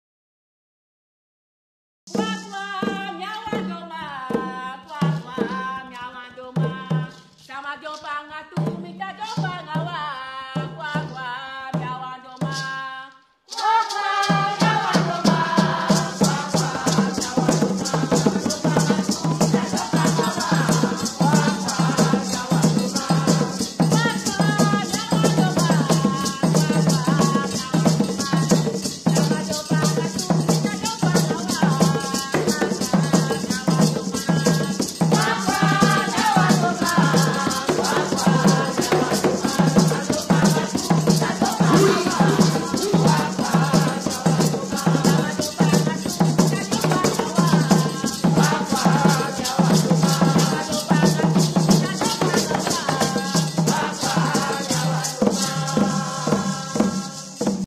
Ampuku (Winti) dance by NAKS Wan Rutu
Performed by NAKS Wan Rutu / Production by Fawaka Creations

The Winti religion is an African based religion in Suriname.
The Ampuku Winti is a nature spirit of the Forest Pantheon.
He has all the knowledge of the different herbs in the forest.